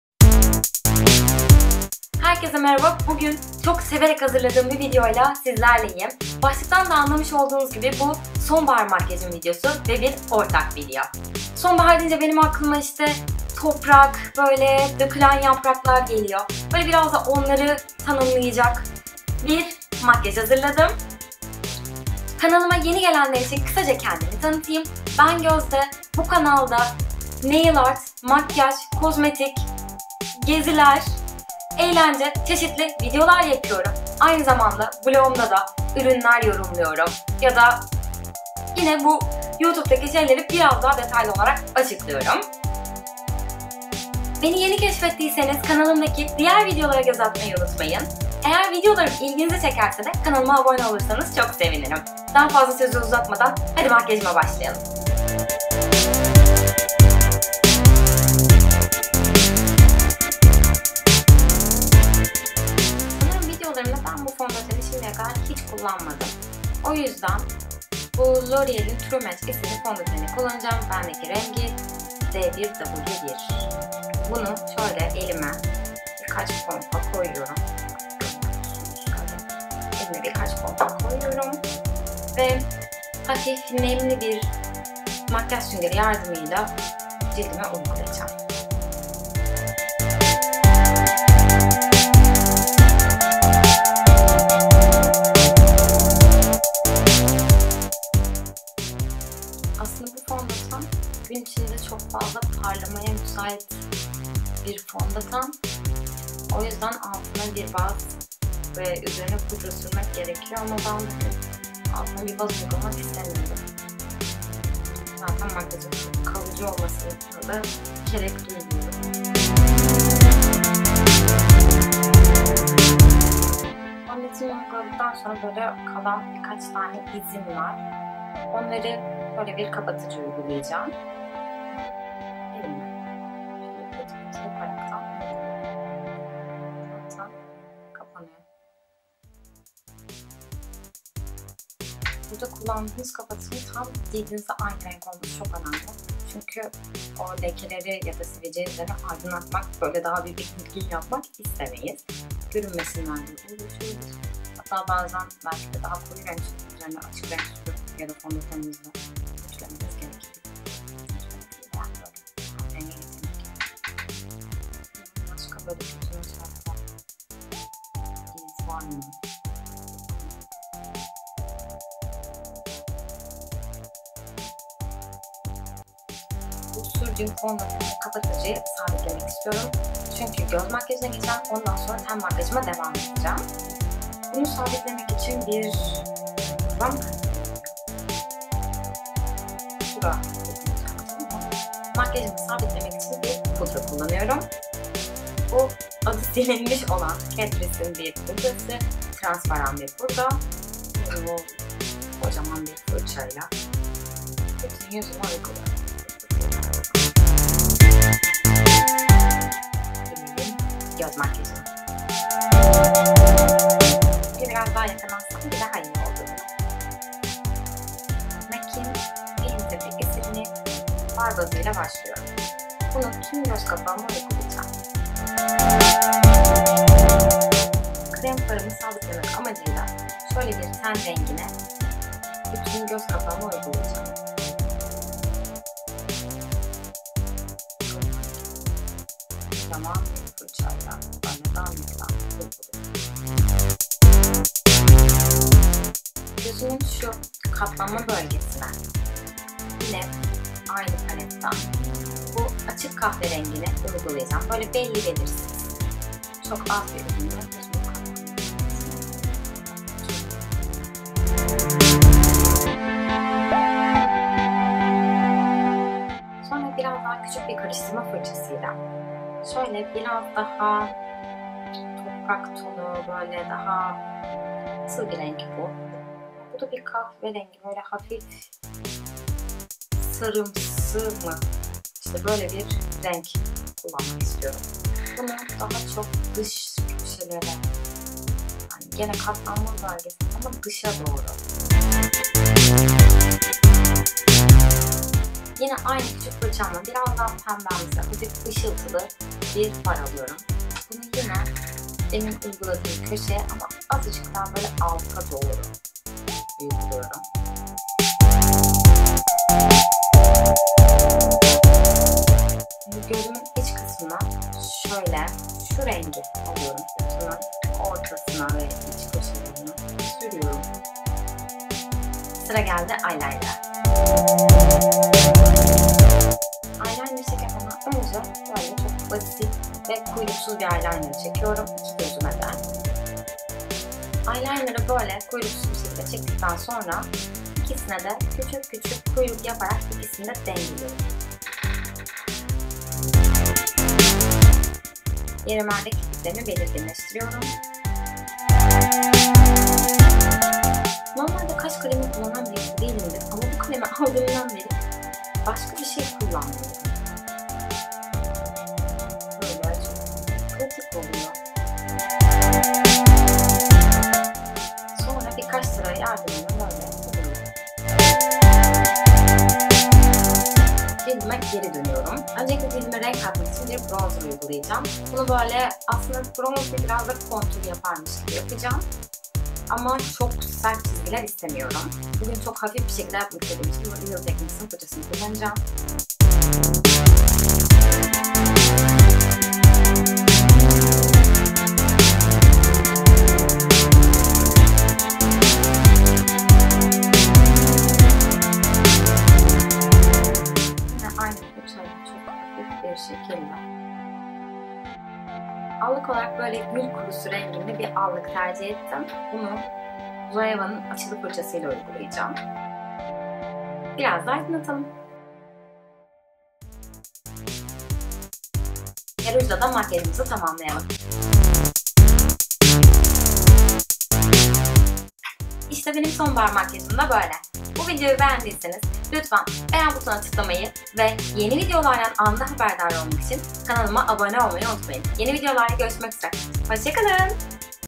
Favori Sonbahar Makyajım (Fall Makeup) - Ortak Video | Gözde Okul
Mevsimlerden sonbahar gelince makyajlarda da biraz değişiklik yapma zamanı geldi. Sonbahar dediğimizde daha turuncumsu toprak tonları akla geliyor. Ben de bundan esinlenerek size bir makyaj videosu hazırladım.

Kullandığım ürünler:
Loreal True Match Foundation
Ebelin Präzisions Make Up Ei
Benefit Boiing Concealer
Catrice All Matt Plus Shine Control Powder 010 Transparent
MAC Paintpot Painterly
Real Techniques Deluxe Crease Brush
Makeup Revolution Naked Underneath
Zoeva 220 Luxe Grand Shader
Zoeva 227 Luxe Soft Definer
Zoeva 231 Luxe Petit Crease
Makeup Revolution I Heart Chocolate
Loreal Superliner Perfect Slim Eyeliner
Art Deco Volume Sensation Mascara
Mac Eye Brows Crayon Spiked
Catrice Defining Blush 020 Rose Royce
Zoeva 127 Luxe Sheer Cheek
MAC Soft & Gentle Highlighter
Zoeva 129 Luxe Fan
Makeup Revolution I think you’re amazing lipstick

Ortak video yaptığımız arkadaşlarımın videolarına da göz atmayı unutmayın